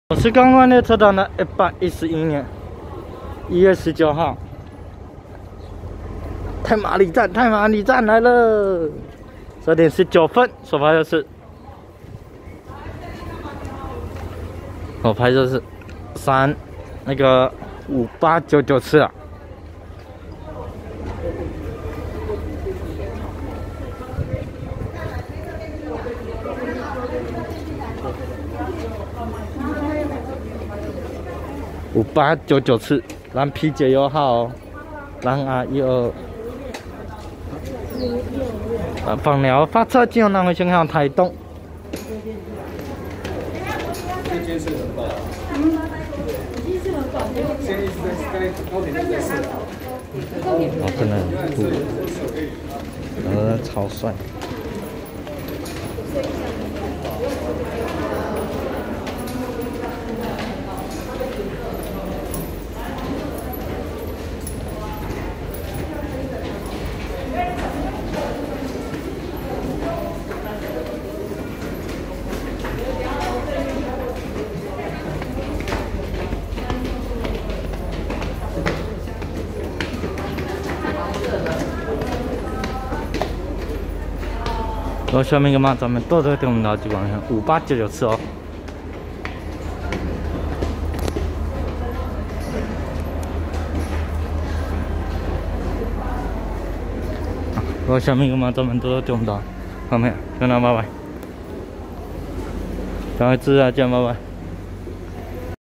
南迴線太麻里站12:19分拍攝5899次枋寮發車經由南迴線開往台東的R122單藍色柴電頭+四節藍皮解憂號，開心為期六天的生日鐵道之旅，來到太麻里就是要去櫻木平交道和看日昇大道
111/1/19南迴線太麻里站12:19分拍攝5899次枋寮發車經由南迴線開往台東的R122單藍色柴電頭+四節藍皮解憂號，開心為期六天的生日鐵道之旅，來到太麻里就是要去櫻木平交道和看日昇大道